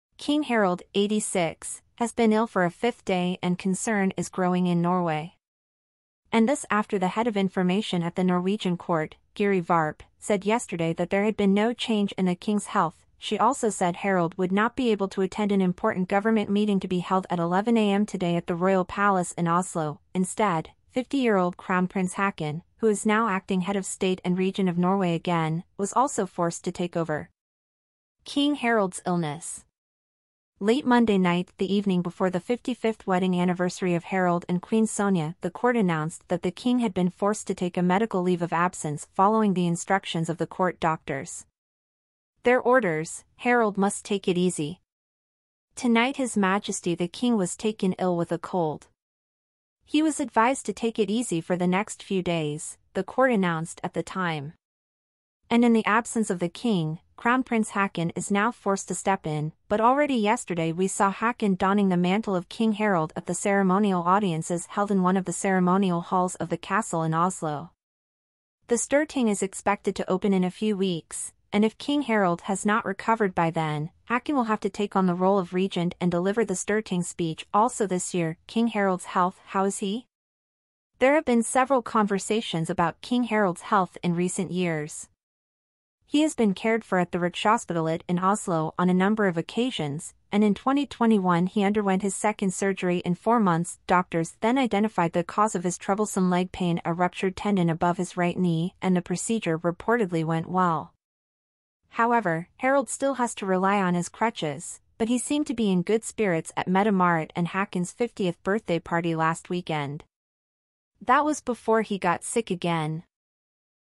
A new court statement about the ailing King Harald
King Harald, 86, has been ill for a fifth day and concern is growing in Norway.

And this after the head of information at the Norwegian court, Guri Varpe, said yesterday that there had been no change in the king's health.She also said Harald would not be able to attend an important government meeting to be held at 11:00 a.m. today at the Royal Palace in Oslo.Instead, 50-year-old Crown Prince Håkon, who is now acting head of state and regent of Norway again, was also forced to take over